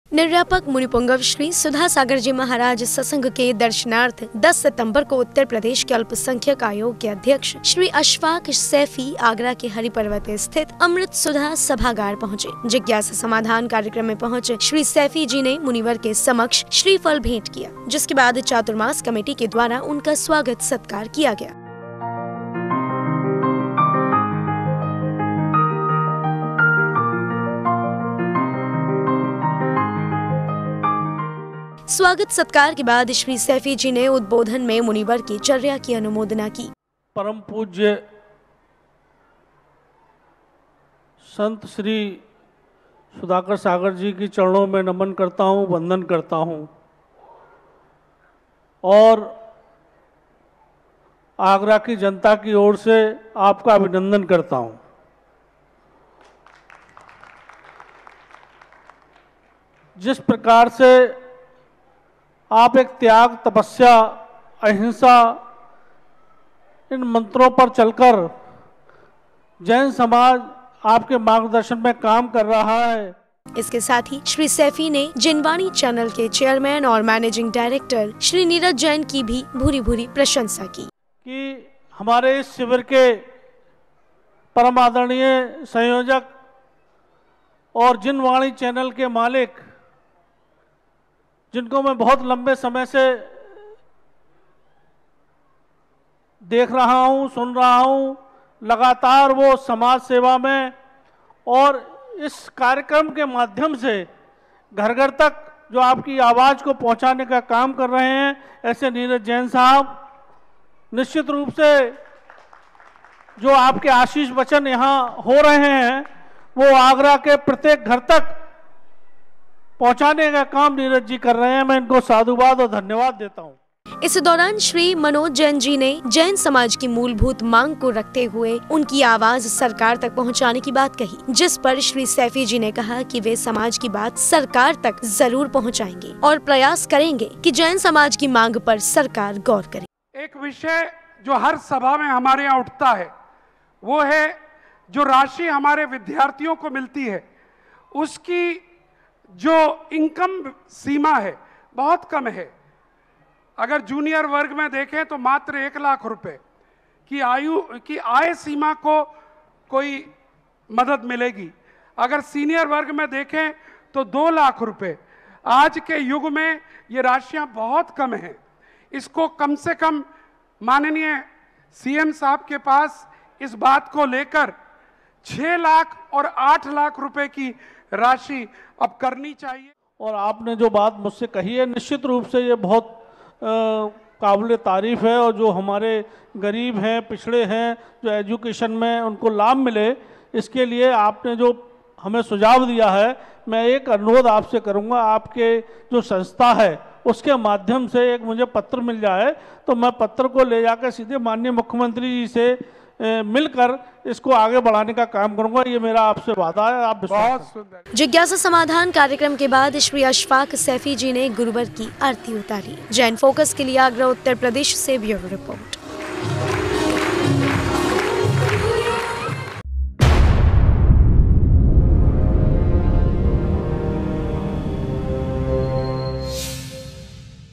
अशफ़ाक़ सैफ़ी ने किए निर्यापक मुनिपुंगवश्री के दर्शन | Agra News | JAIN FOCUS
अशफ़ाक़ सैफ़ी ने किए निर्यापक मुनिपुंगवश्री के दर्शन
- उत्तर प्रदेश अल्पसंख्यक आयोग के अध्यक्ष हैं श्री अशफ़ाक़
- जैन समाज की मूलभूत मांग की बनेंगे आवाज़ - श्री सैफ़ी
- संध्या काल में उतारी निर्यापक मुनिपुंगवश्री की आरती